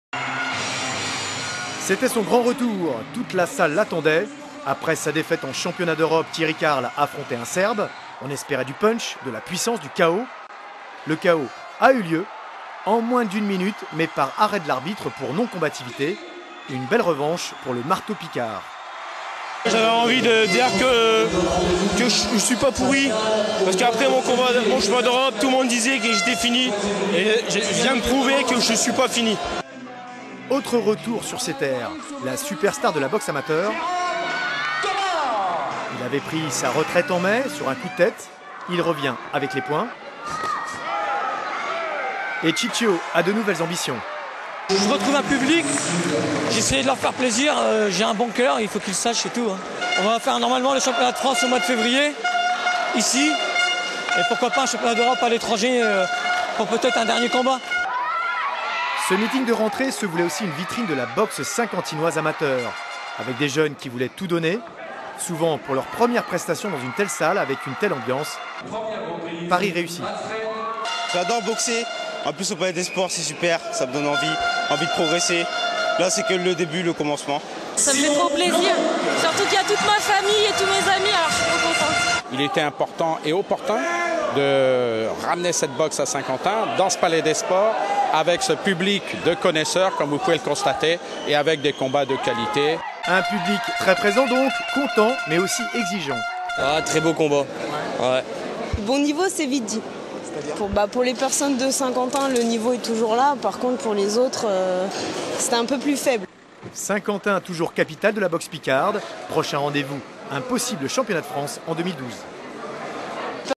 MS_GALA_BOXE_ST_1523560_NQP0EJRL.wmv
Gala de boxe à Saint-Quentin le 2 décembre 2011, avec des champions picards Thierry Carl et Jérome Thomas en pleine forme... Un retour ? et de jeunes espoirs Alfred Wande Wula et Pauline Leconte. Reportage sur une belle soirée du noble art. arnaud wust julien lanchas